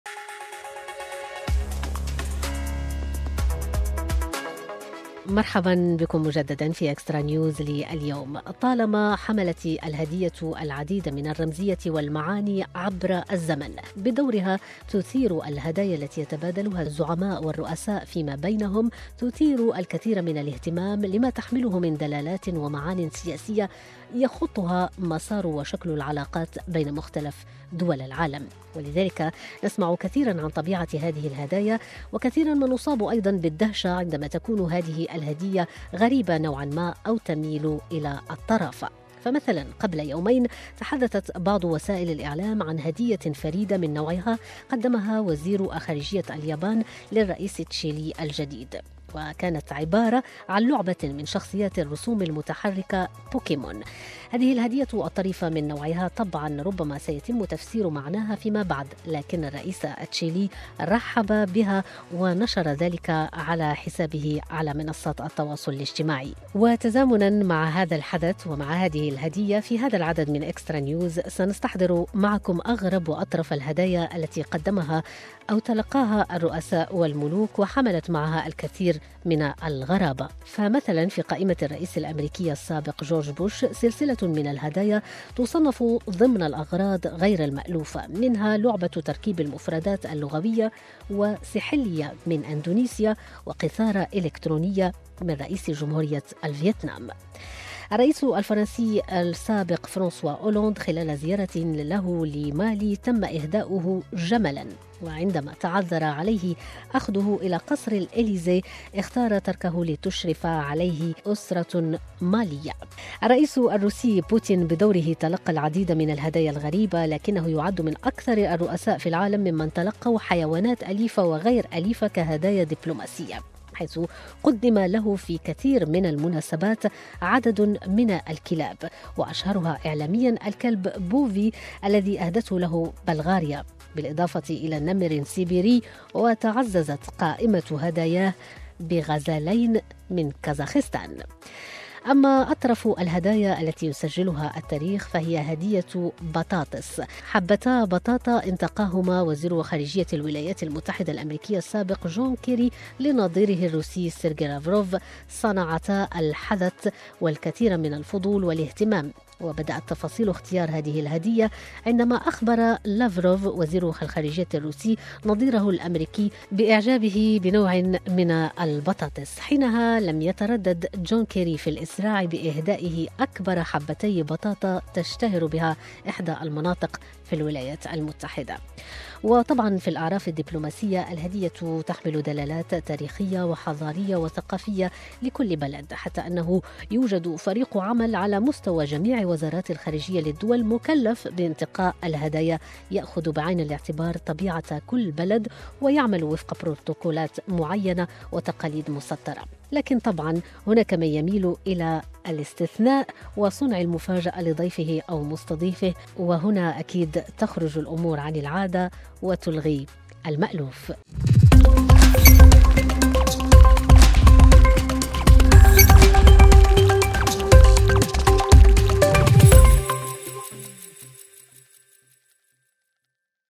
أغرب الهدايا التي تلقاها الرؤساء والزعماء
تثير الهدايا التي يتبادلها الرؤساء و#الملوك بين بعضهم البعض أو يتلقاها الزعماء الكثير من الاهتمام، لما تحمله من دلالة ومعانٍ سياسية يخطها مسار وشكل العلاقات بين مختلف دول العالم .